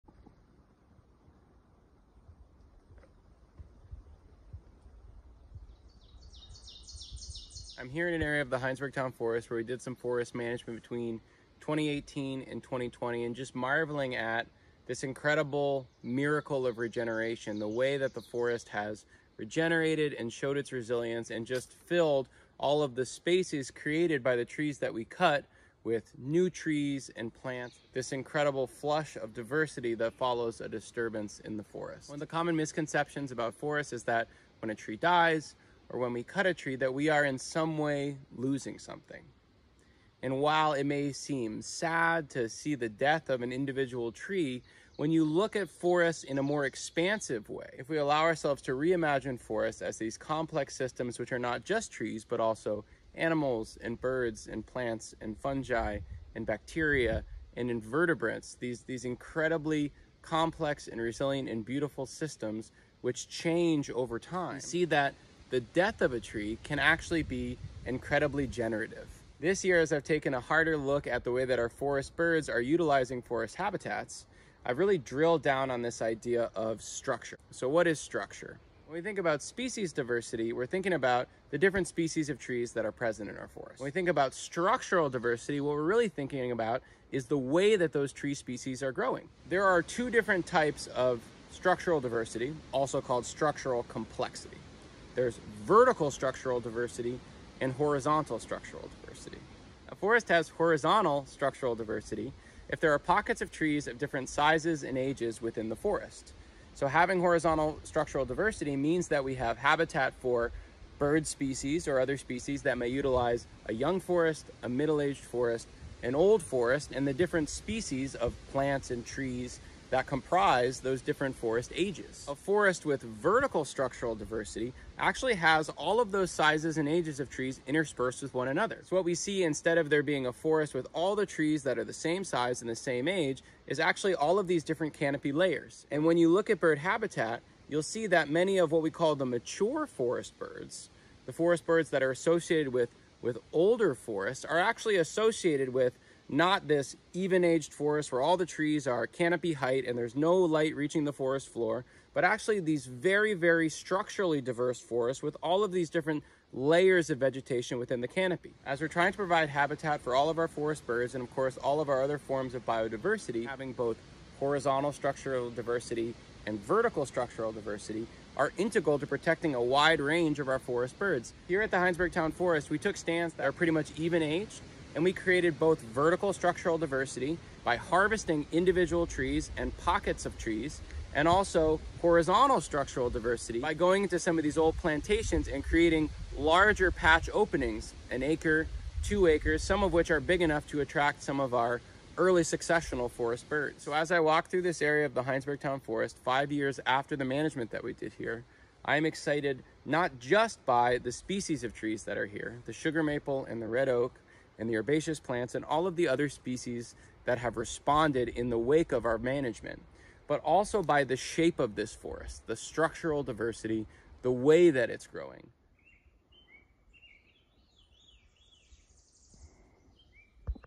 What is Structural Diversity in a Forest?
What is structural diversity? In this video, Ethan visits an area of the Hinesburg Town Forest that he managed 5 years ago and finds that it has regenerated into a diverse, complex multi-aged forest. He talks about what structural diversity is, the difference between horizontal structural diversity and vertical structural diversity, and the benefits of structural diversity to biodviersity -- especially to songbirds.